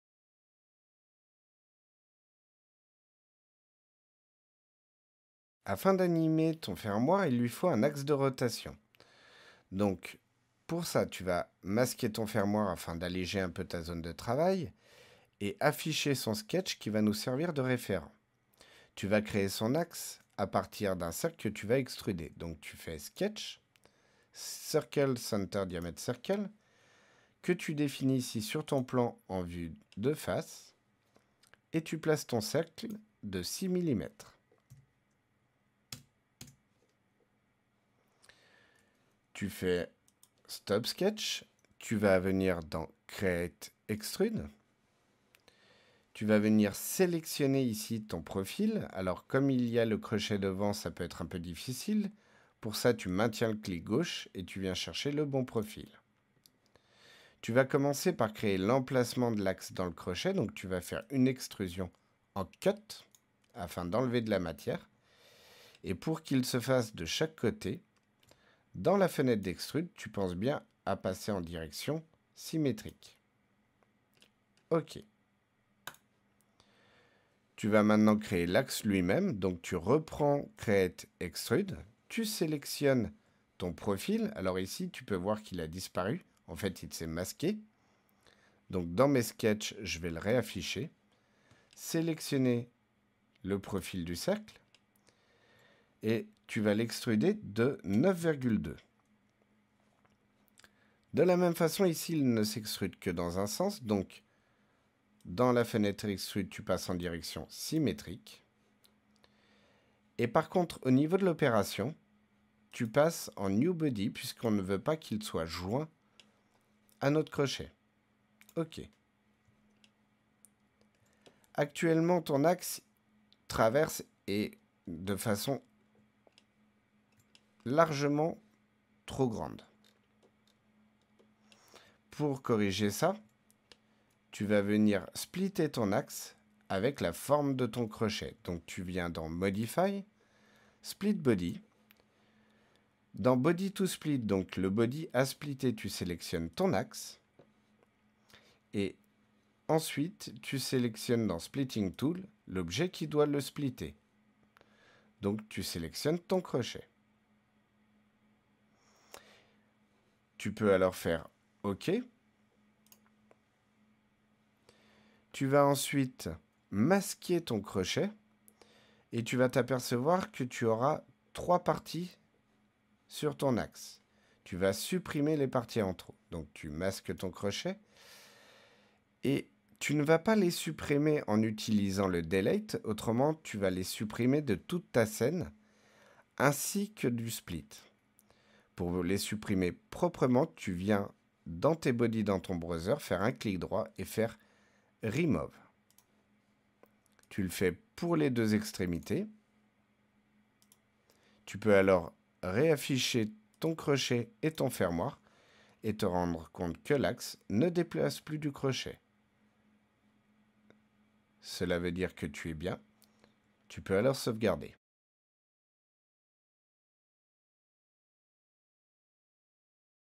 Exercice - Crochet - La montre à Gousset EX6.3.5
Dans cette vidéo, vous allez voir comment animer le fermoir à partir d'un axe de rotation avec différents outils comme l'extrusion et l'outil scinder le corps.

Pour plus d’informations sur nos solutions de formation et d’accompagnement à l’intégration de technologies et compétences 3D, rendez-vous sur nos sites

Rejoignez-nous !